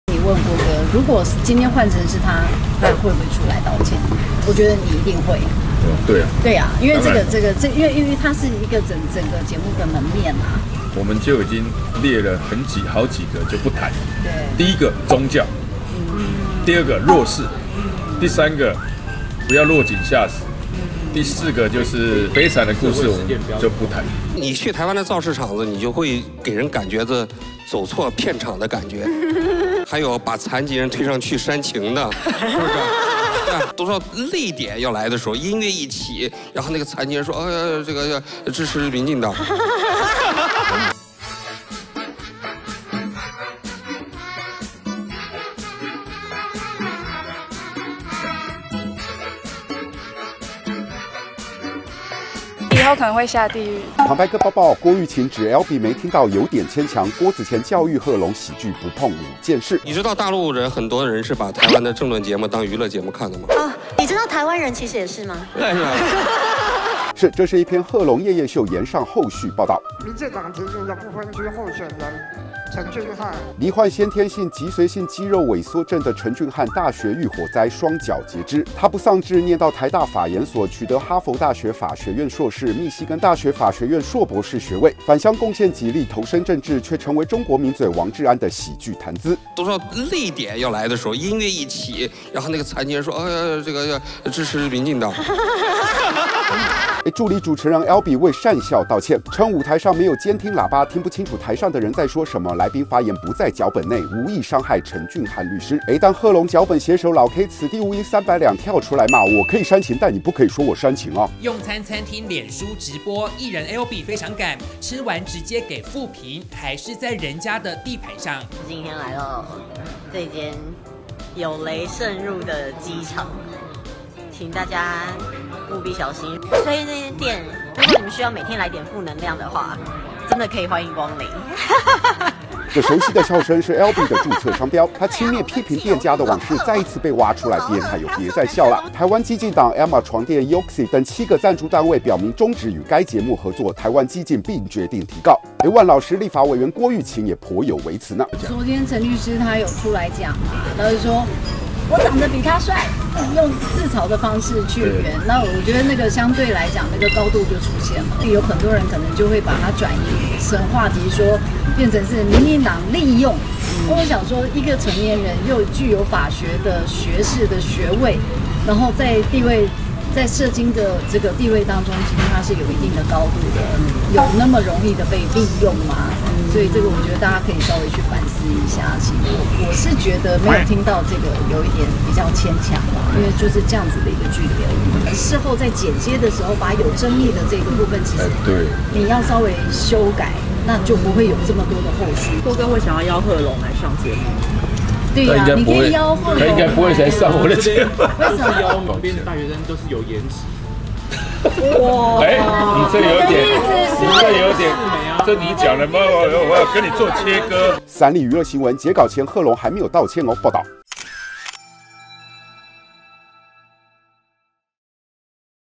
郭昱晴指Albee沒聽到「有點牽強」 郭子乾教育賀瓏喜劇不碰5件事｜三立娛樂星聞
★訂閱【女孩要幹嘛】甜寵浪漫戲劇看到飽